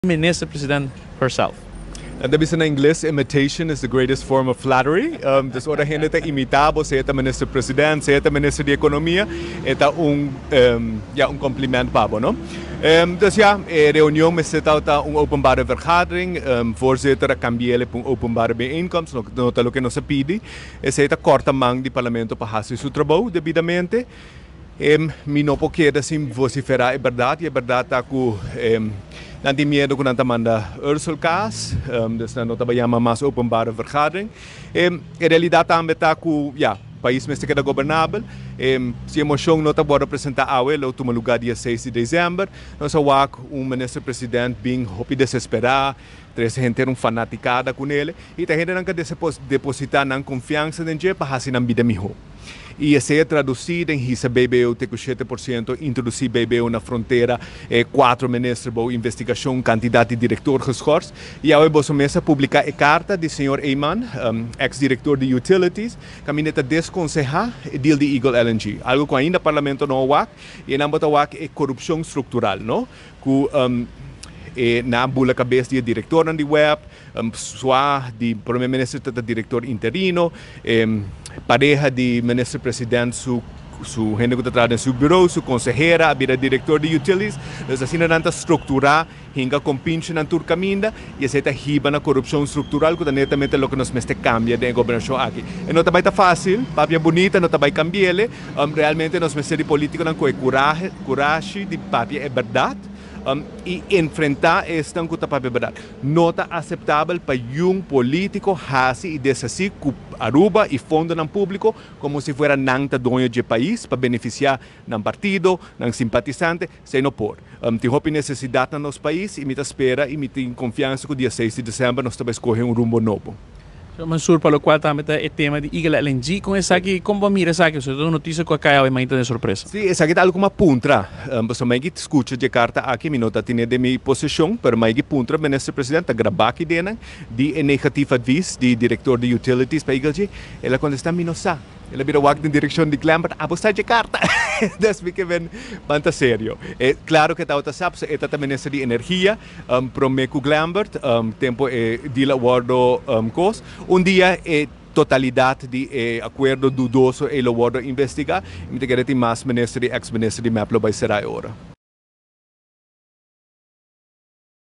Miguel Mansur (Accion21): Presidente di Staten a corta man di parlamentario p’e cambio di reunion
Durante e encuentro publico di Parlamento, manera nos a indica caba anteriormente, tabatin basta controversia. Hasta por menciona cu e palabra "patetico" a sali for di boca di e Prome Minister.